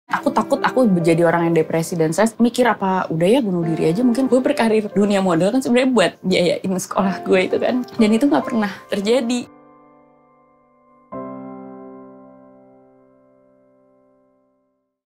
Dian Sastrowardoyo Menemukan Makna Hidupnya - TEASER
Temukan cerita Dian Sastrowardoyo dalam menemukan makna hidup di Dian melewati banyak hal dan memetik banyak pelajaran untuk dirinya menjadi lebih baik lagi. Dari semua rintangan yang pernah Dian hadapi, ada mimpi-mimpinya yang belum ia capai yang membuat Dian memaknai hidup dengan menyebarkan kebaikan dan kebermanfaatan bagi wanita Indonesia melalui beasiswa Dian.